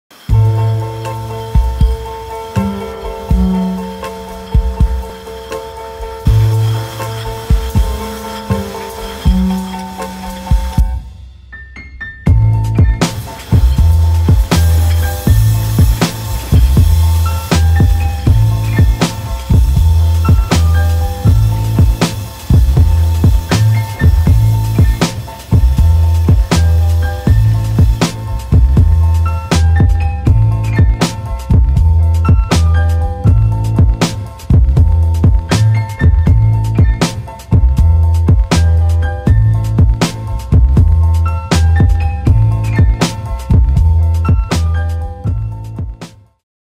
DJI RONIN M X SONY A6300
CINEMATIC TEST OF THE SONY A6300 FLYING ON THE DJI RONIN M. CINEMATIC DJI Ronin M test. Cinematic Sony a6300 test.

UCO.media